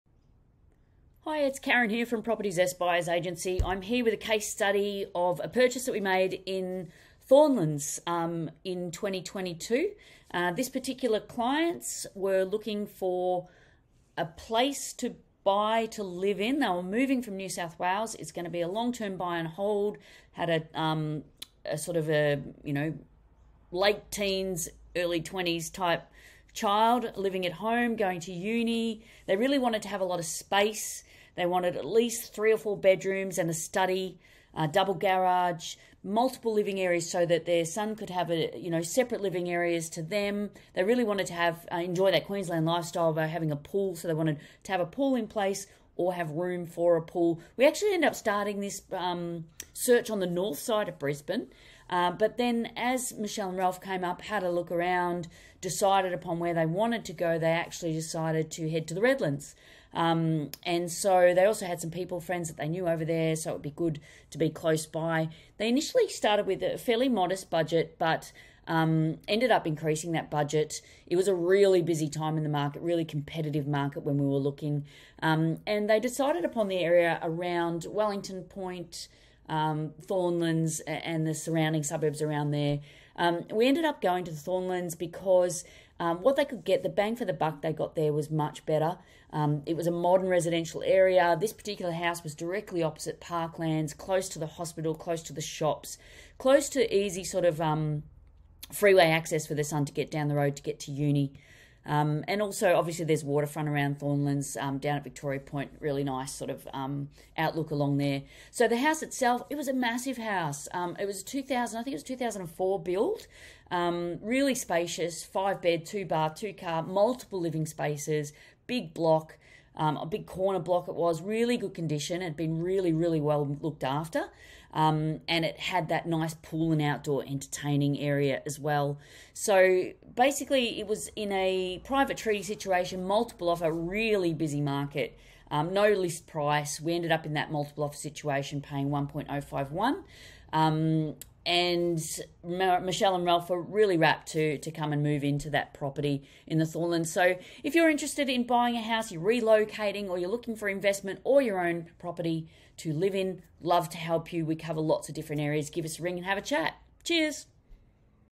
BUYERS AGENT CASE STUDY - THORNLANDS, REDLANDS
- Brisbane Buyers Agent and Sunshine Coast Buyers Agent - This PPR had it all from our buyers moving from NSW - 5bd, 2bth, double garage, multiple living areas, pool and outdoor entertaining area!